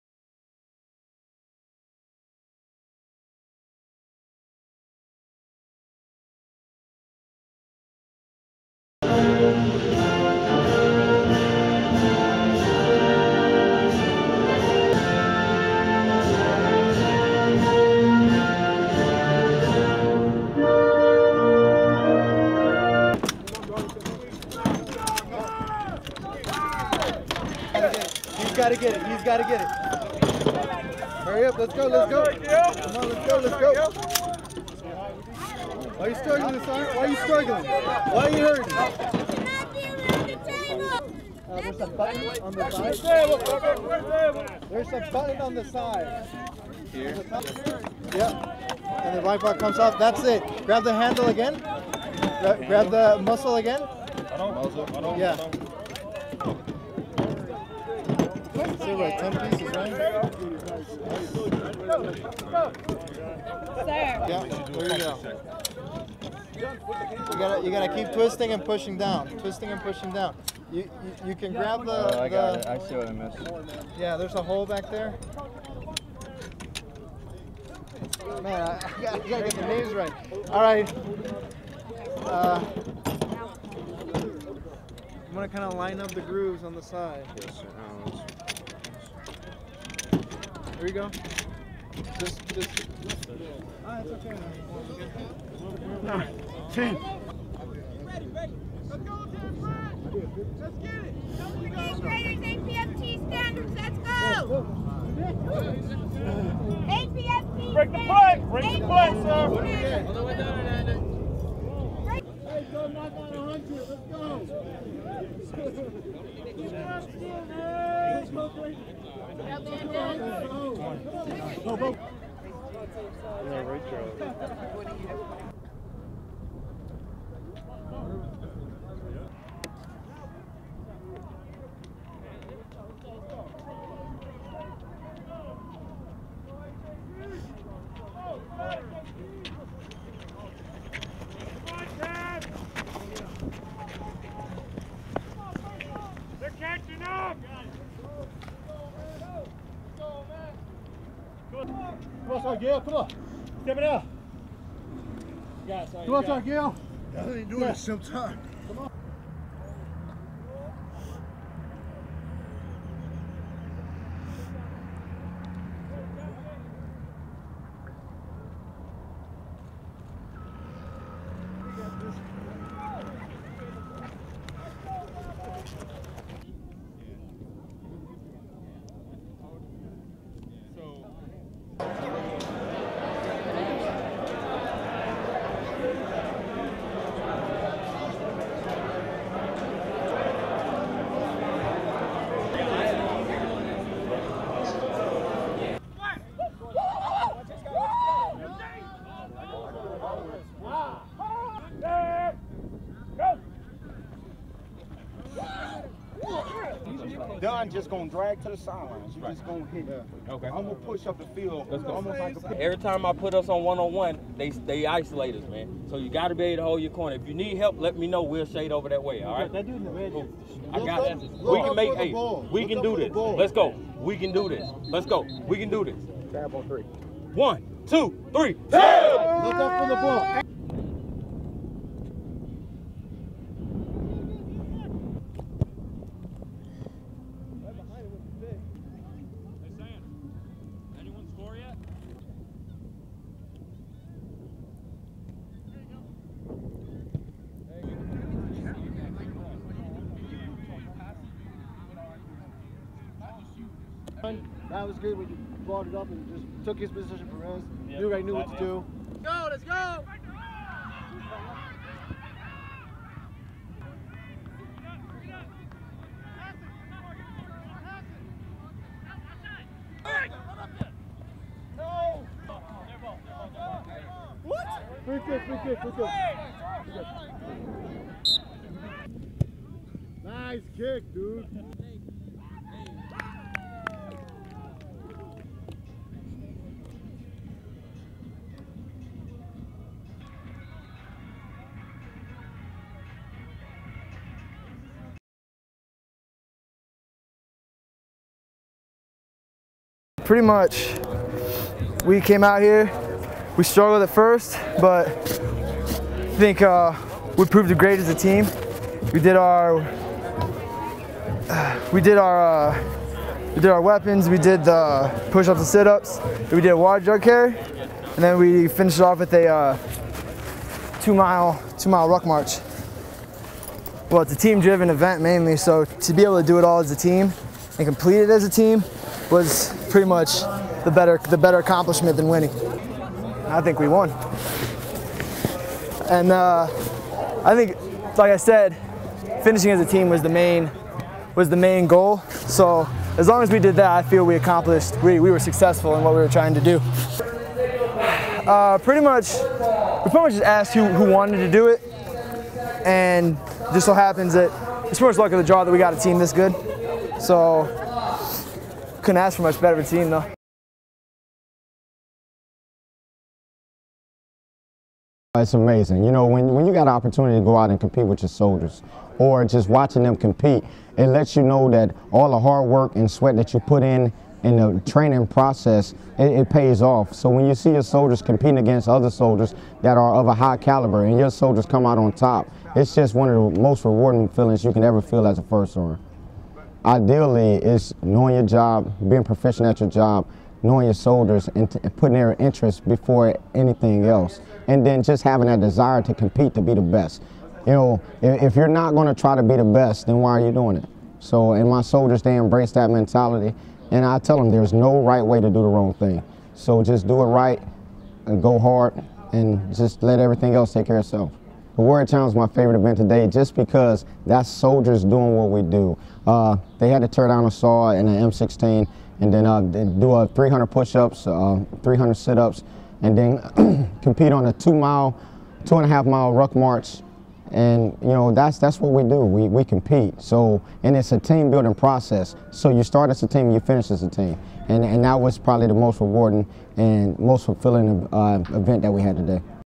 2-12 FA Org Day
Footage of Soldiers from 2-12th Fires Brigade competing against each other in a variety of events. (soccer, football, and a warrior challenge) The Warrior Challenge included Soldiers taking apart and putting weapons back together, push ups and sit ups, as well as a ruck march. There are slated interviews at the end of the b-roll. Also available in high definition